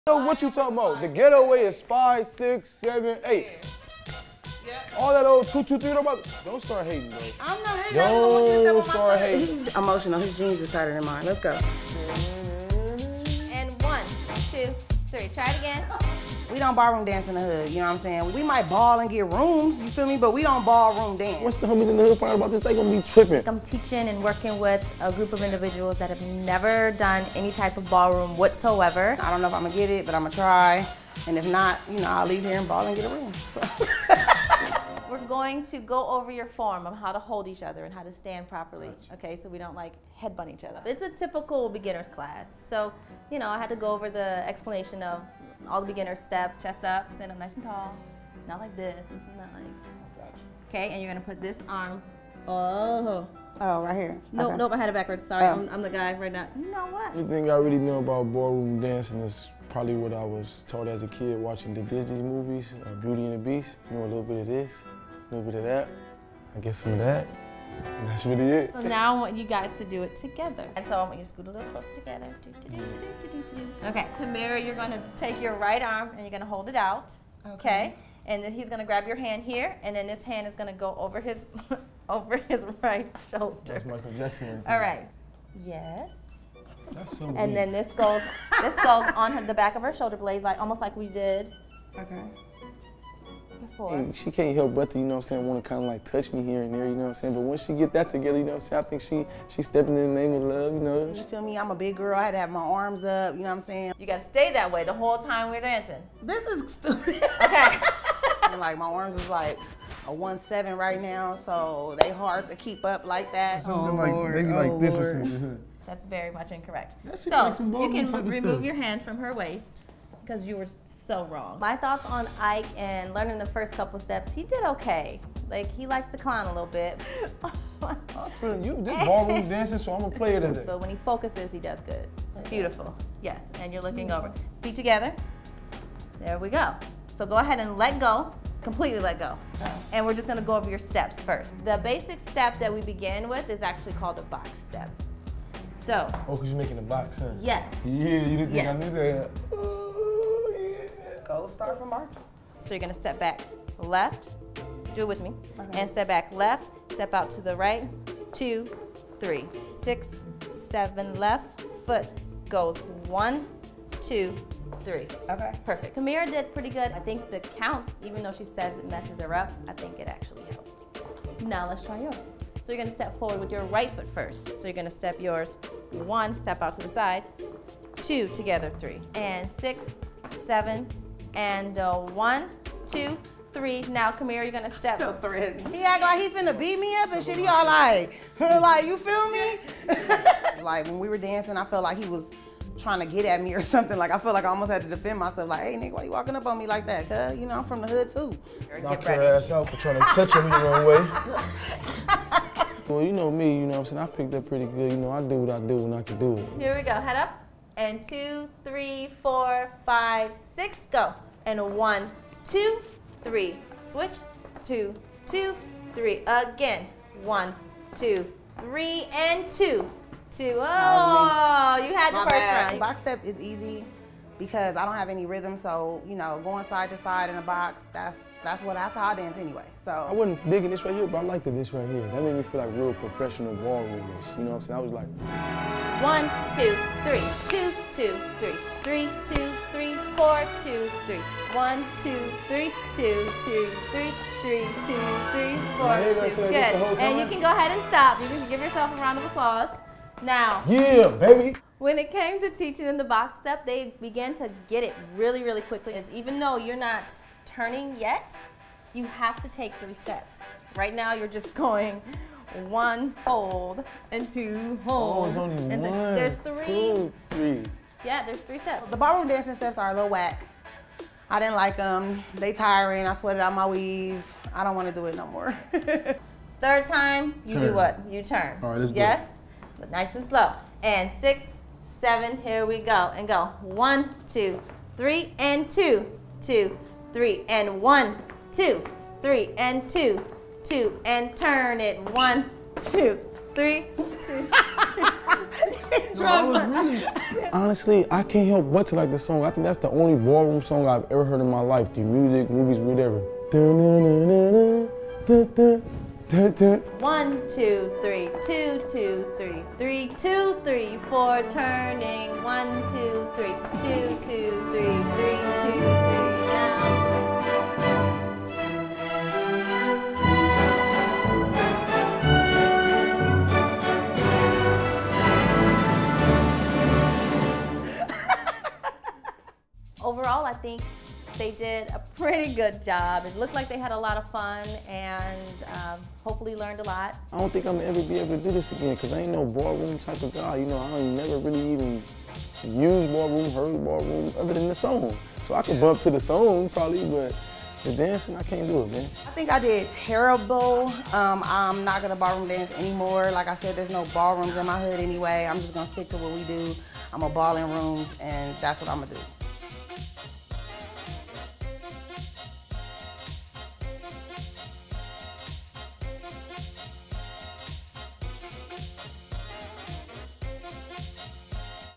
Comedians Try Ballroom Dancing 'For the First Time' | All Def Comedy
What happens when you take comedians and put them in a ballroom where making jokes won't benefit their footwork? Watch as Kamira White and Ike Williams try to master the fine art of ballroom dancing.